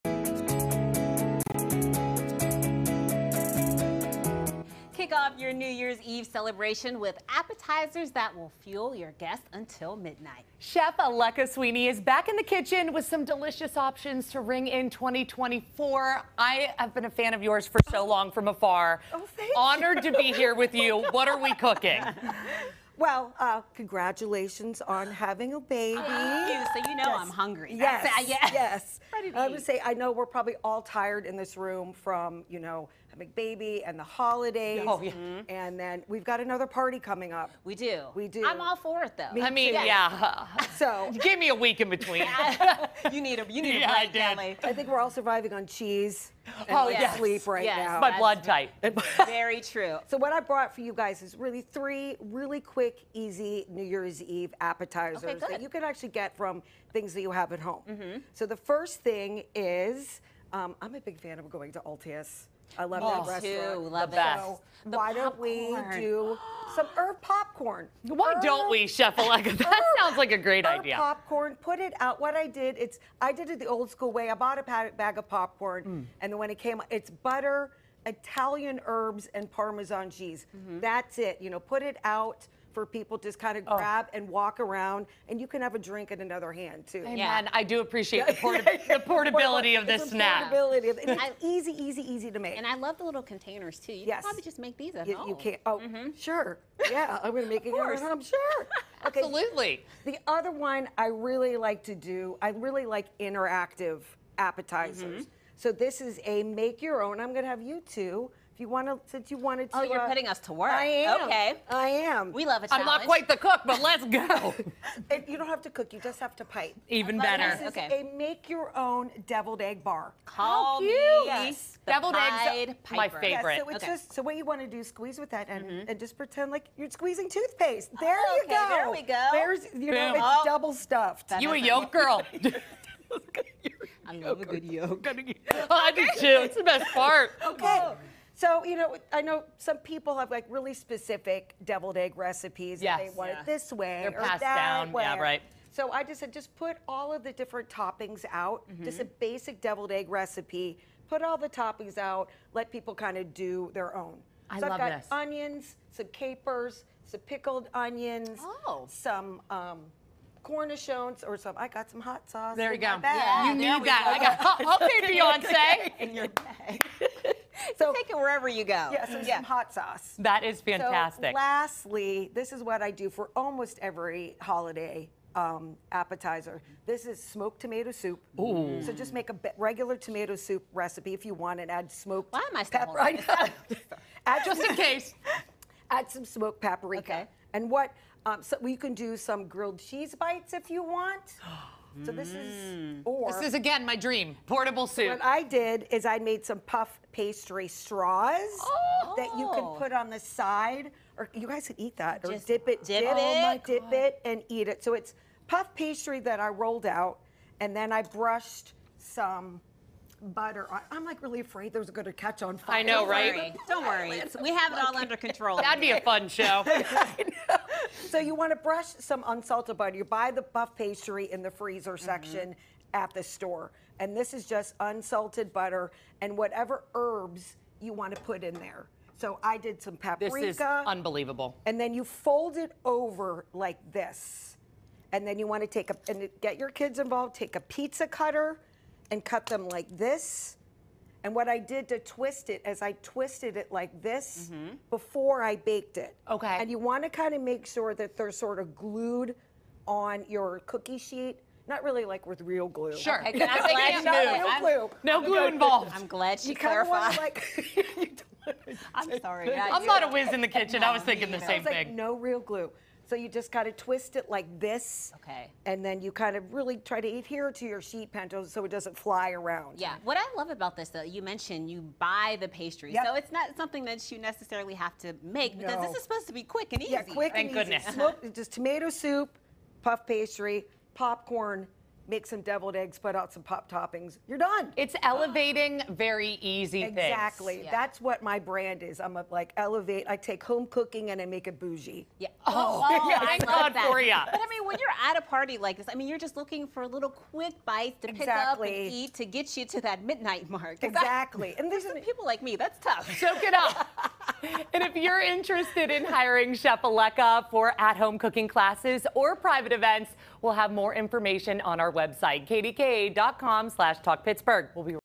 Chef Alekka Sweeney shows us how to whip up the best bites for New Year's Eve
Kick off your New Year's Eve celebration with appetizers that will fuel your guests until midnight. Chef Alekka Sweeney is back in the kitchen with some delicious options to ring in 2024.